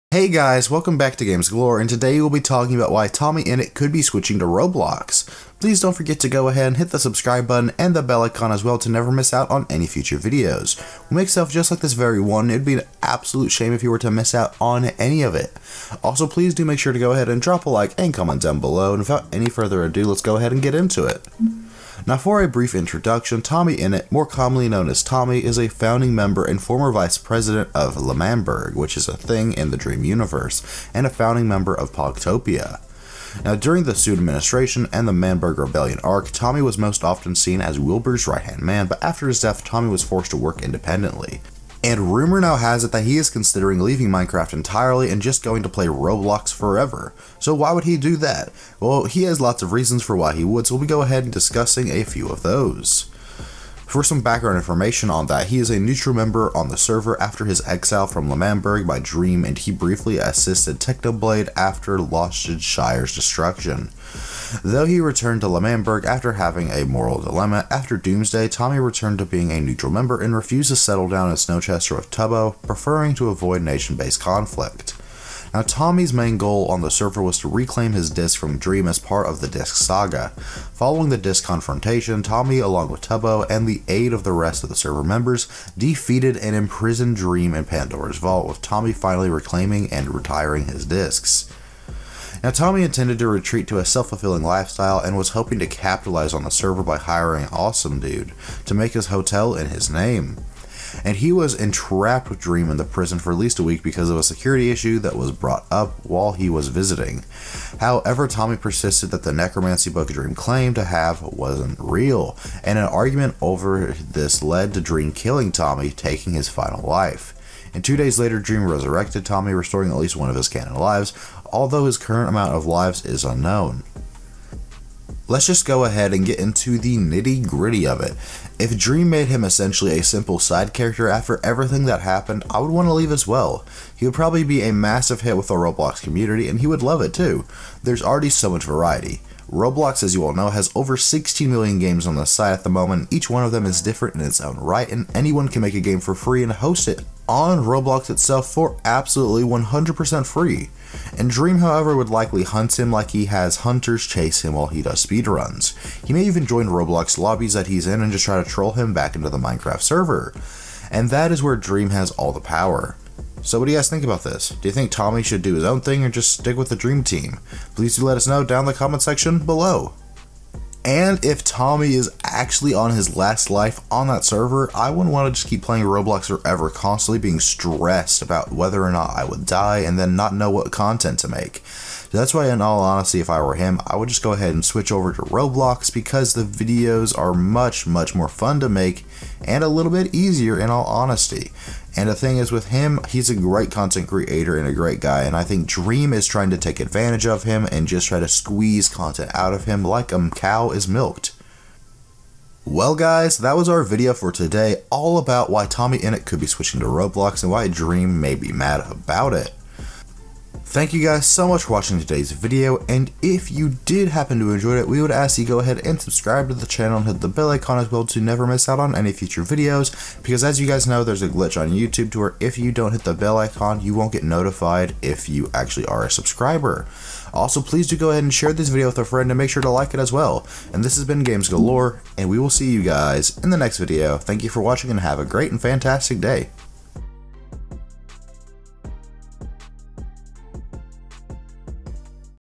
Why TommyInnit could be SWITCHING to ROBLOX! Dream is MAD!
Why TommyInnit could be switching to Roblox soon and why Dream is mad! Tommyinnit dream smp is still friends with Technoblade. But what will happen with Wilbur?